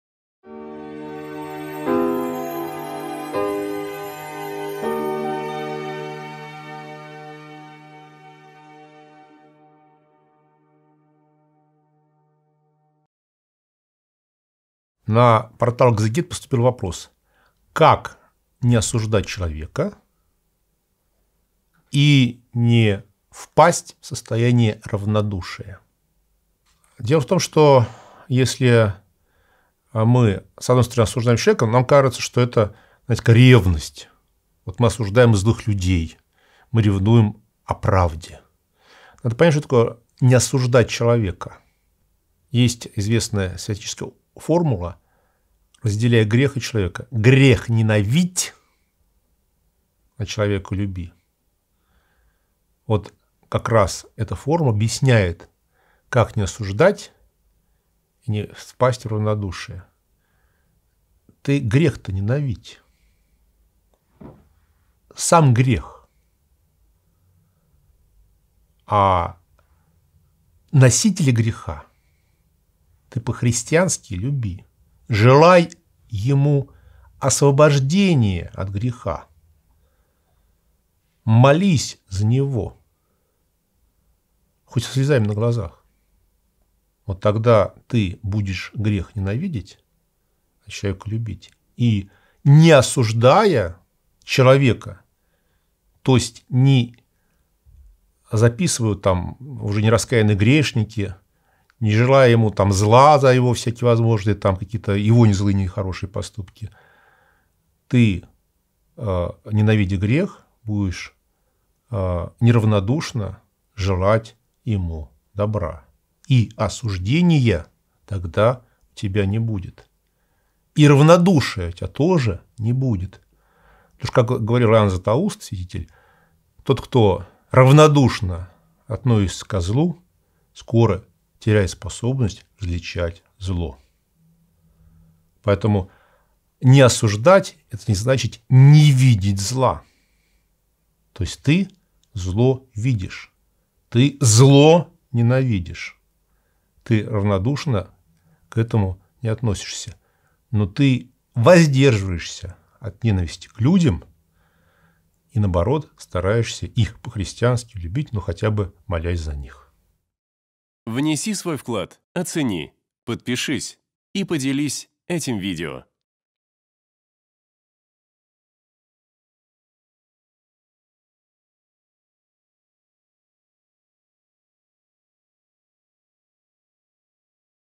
Как не осуждать человека, но при этом не быть равнодушным ? Протоиерей Александр Никольский
Команда Экзегет.

В сегодняшнем видео на библейском портале Экзегет вместе с протоиереем Александром Никольским отвечаем на вопрос: "Как не осуждать, но при этом не быть равнодушным?"

🔸  Разделять грех и человека
🔸  Ненависть ко греху
🔸  Желание добра
🔸  Молитва